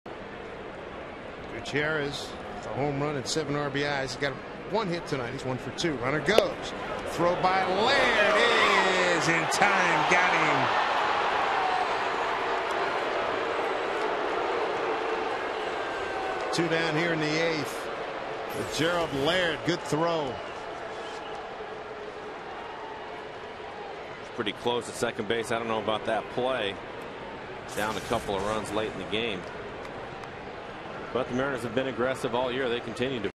2009/04/18 Laird's strong throw
DER@SEA: Laird catches Johnson trying to steal second